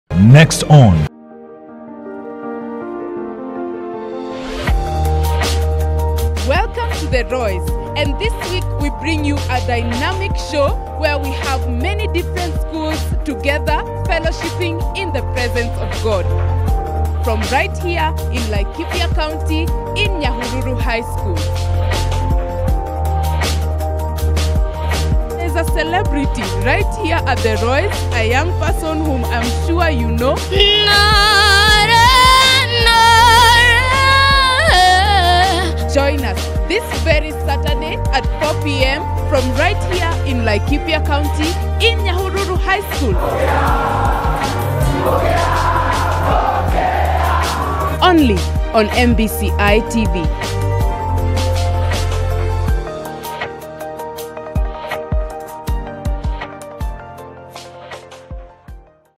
Rally: Nyahururu High School - Promo// The R.O.Y.S - For Christ International
Welcome to The R.O.Y.S - For Christ International.

Schools usually have Weekend Challenges which are culminated by a Grand Rally. In the Rally, we invite other schools to fellowship together with us as we relish in the Presence of God.

This is the Nyahururu High School Weekend Challenge Rally. We had a GREAT time with these young people. We were joined by Magomano Girls' High School & Busara Forest View Academy. Enjoy this 1 minute promo as we await the upload of the full episode soon and very soon.

CELEBRITY ALERT!!! We have a young celebrity on this show. Tell us whether you have recognised her in the comments.

Always praying for you.
Shalom.

Facebook: The R.O.Y.S - For Christ Intl